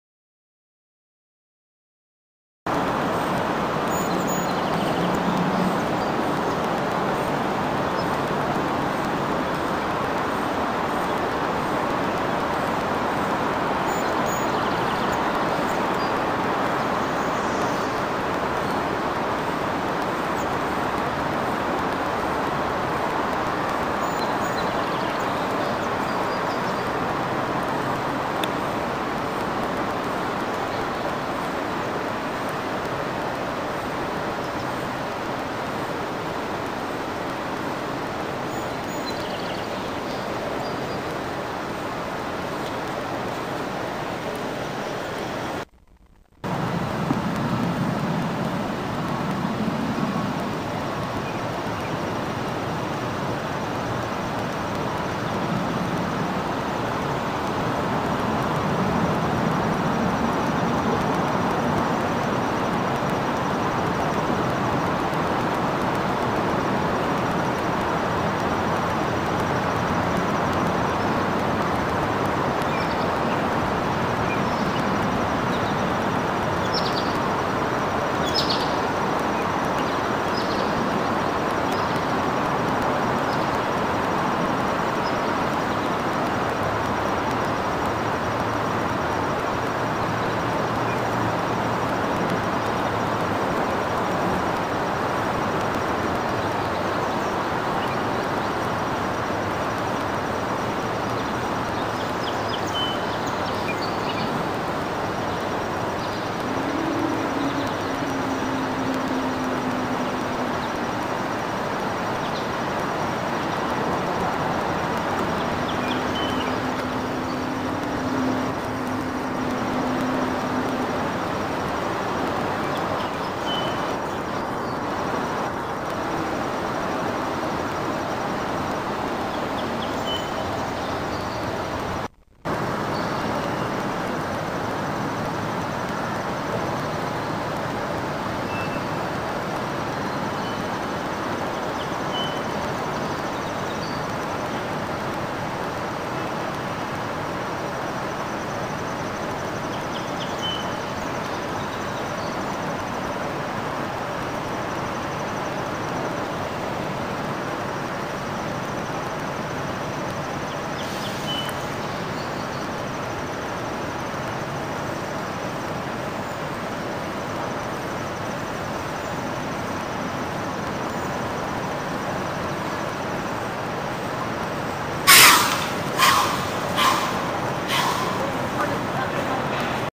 198-Aug 11/14 - doe & twins
Doe with her twins in Morningside Park. She was fine until I squatted down to continue filming her. She got agitated